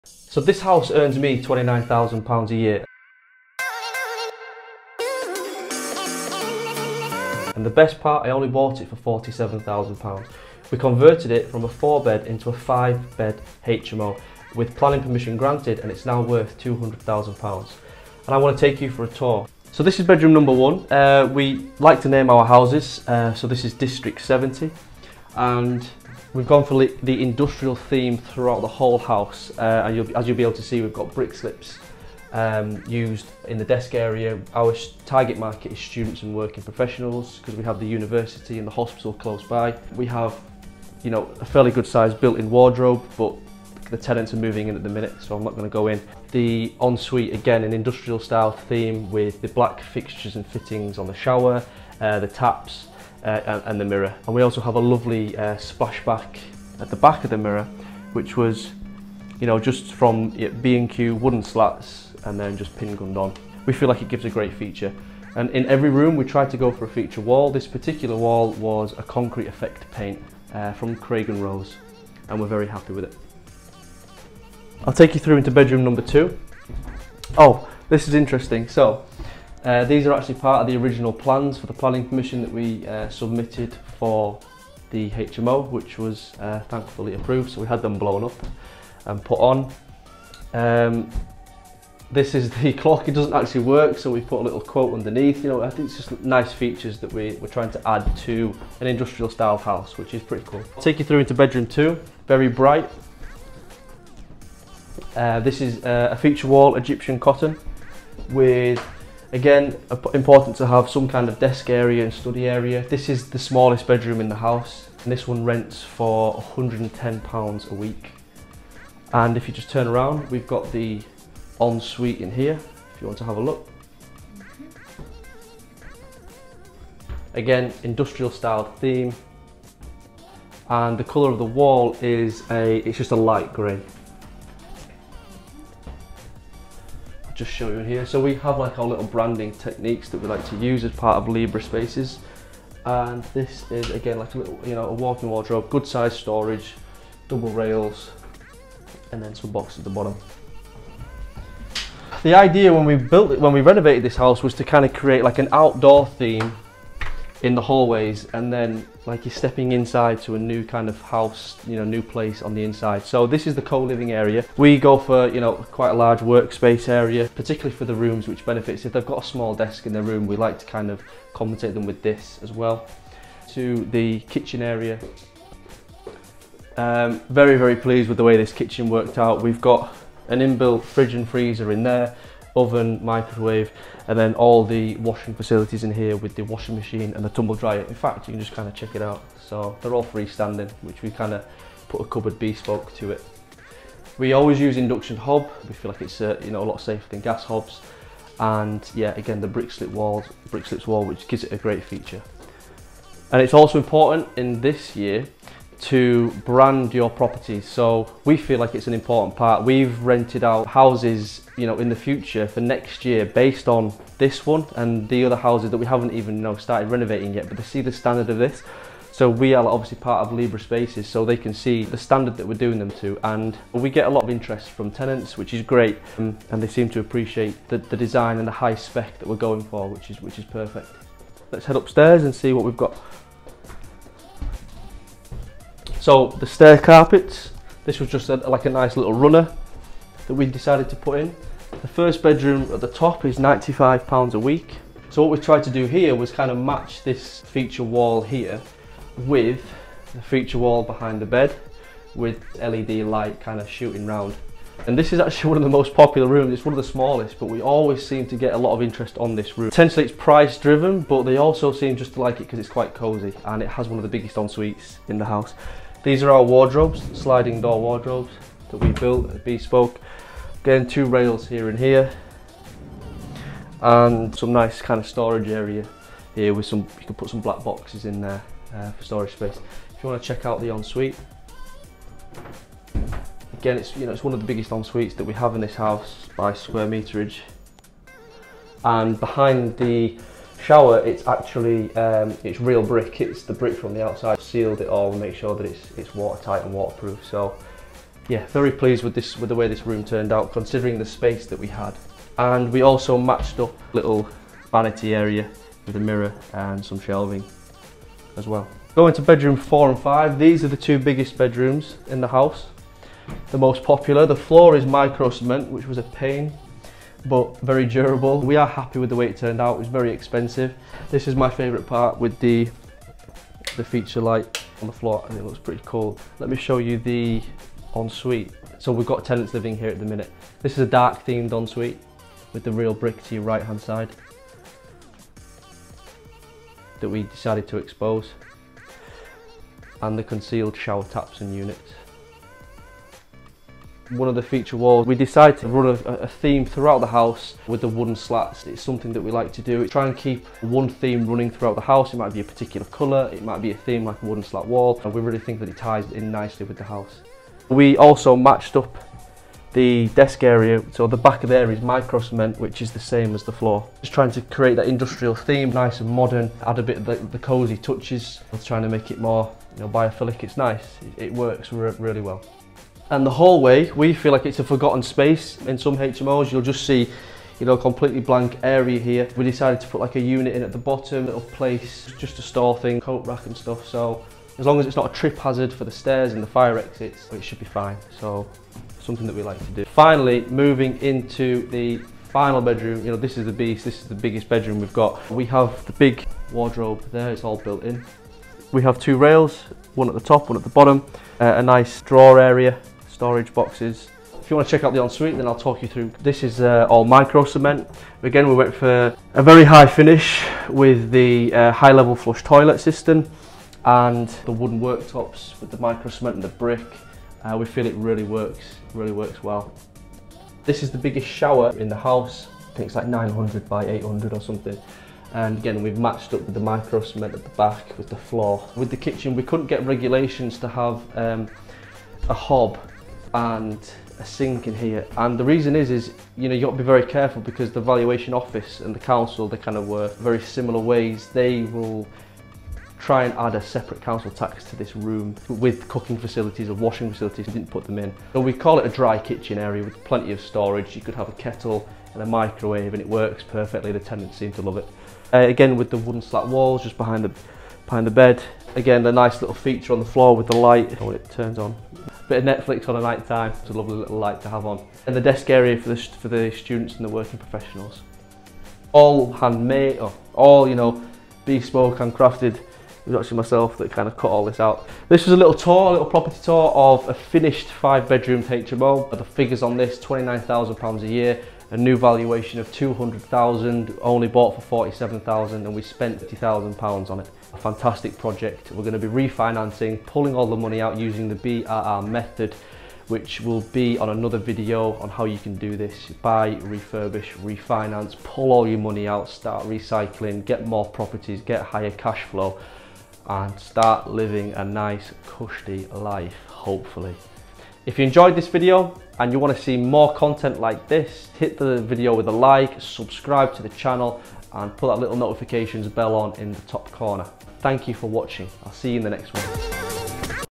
If You Want to Make £29k From an Investment Property - You Need to See This
James Property

In this video I'll be showing you a full house tour of my HMO property which has enabled me to earn £29,000 gross income per year from just this one property investment.

If you're a property investor looking to maximize your returns, then "If You Want to Make £29k From an Investment Property - You Need to See This" by James Property is a must-watch video. In this engaging and informative presentation, James shares his expert insights and strategies for achieving significant returns on your property investments.

Through real-world examples and practical advice, James reveals how you can turn a modest investment property into a significant income stream. He explains the key factors that determine the success of your investments, including location, financing, and property management.

With James' guidance, you'll learn how to identify the right properties, secure financing, and manage your investments for long-term success. You'll also gain a deeper understanding of the property market and how to navigate its various challenges and opportunities.

Whether you're a seasoned investor or just starting out, "If You Want to Make £29k From an Investment Property - You Need to See This" is an essential resource. With James' expert insights and practical tips, you'll be well on your way to achieving significant returns on your property investments.

So don't miss out on this valuable opportunity to learn from one of the industry's top experts. Watch "If You Want to Make £29k From an Investment Property - You Need to See This" by James Property today and start building your own successful property portfolio.